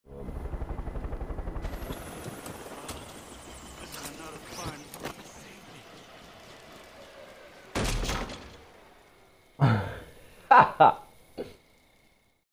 How not to land A Helicopter.
The wrong Button can often be fatal, Luckily I wasn't too high off the ground and I Managed to survive via parachute somehow.

if watching isn't enough or you. we can rumble here and Monetize your content now! Free!

Sign up for the Pi crypto network HERE!